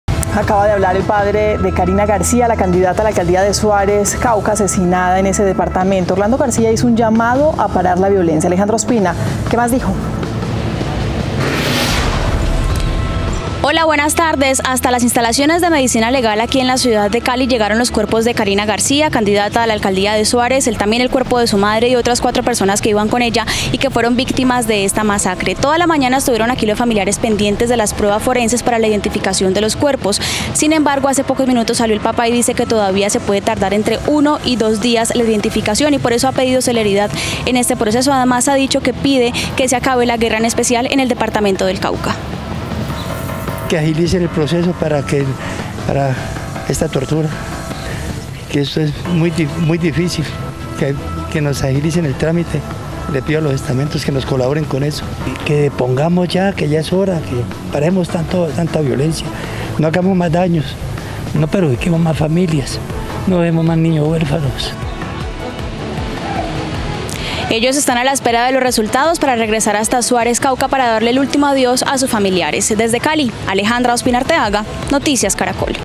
Habló papá de Karina García desde Medicina Legal en Cali, a donde se llevaron víctimas de la masacre
Familiares de la candidata a la Alcaldía de Suárez y las otras cinco personas asesinadas en Cauca esperan los resultados forenses y la entrega de los cuerpos.